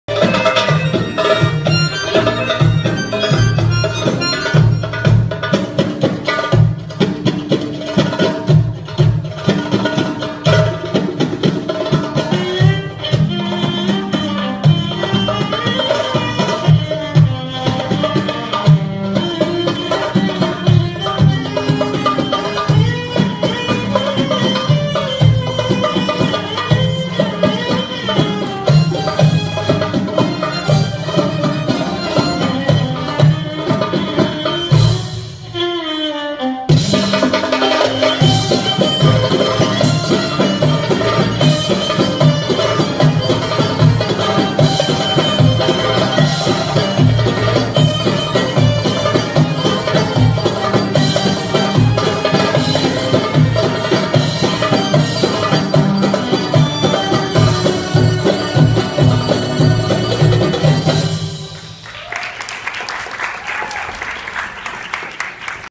Janah Spinelis - BD FACTOR 2016. - clássica
Etapa 1 da competição Belly Dance Factor.  Rotina clássica Oriental.